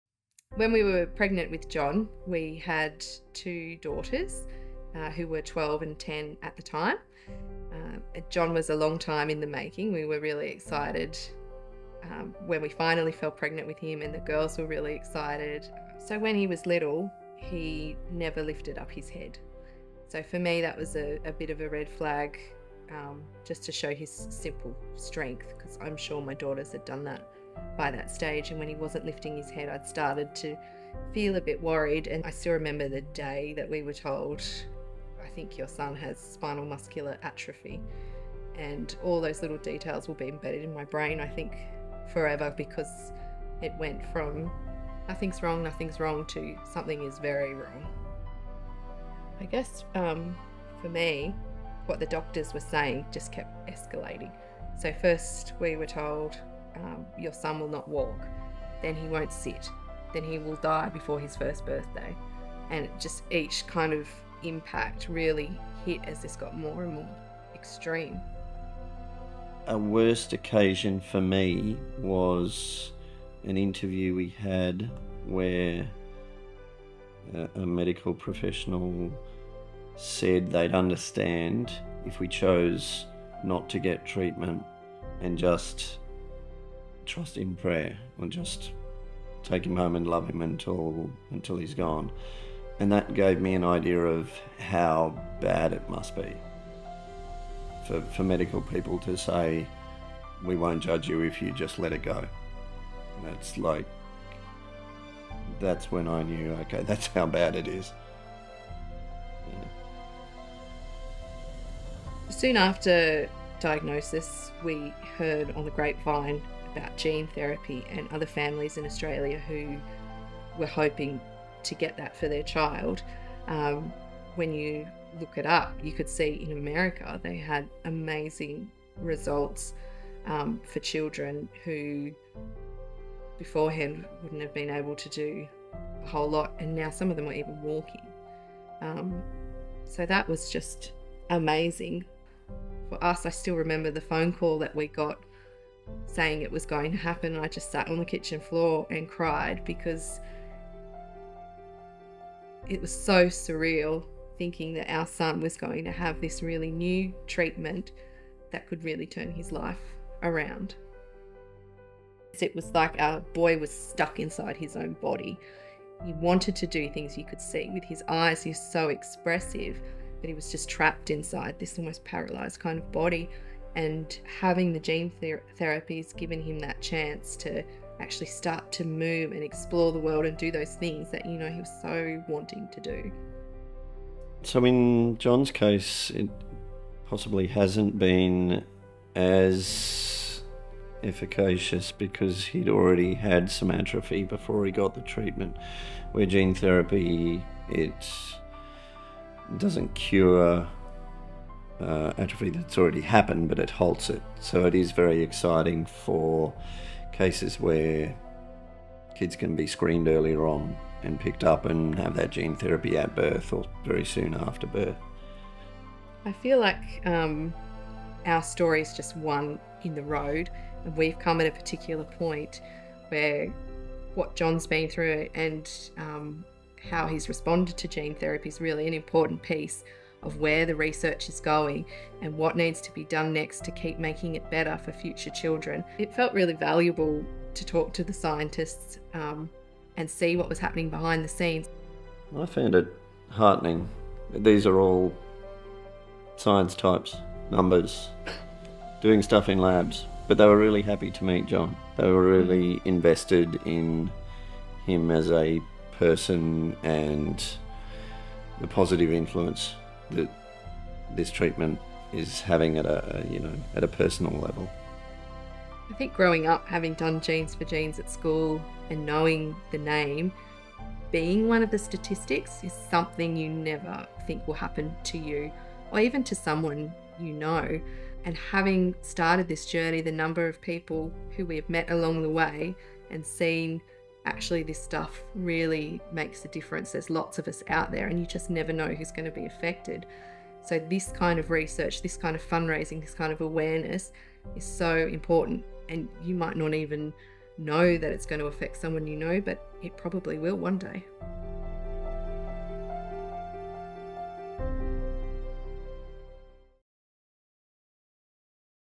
Meet Jon | 2024 Face of Jeans for Genes
You can help our scientists find cures for genetic diseases so kids like Jon can go back to what they do best – be kids.

0:00 The beginning of Jon's journey
1:10 Where all hope felt lost
1:52 Gene therapy brings hope to Jon and his family
3:36 Jon's impact to our medical research
4:26 Why Jeans for Genes is so important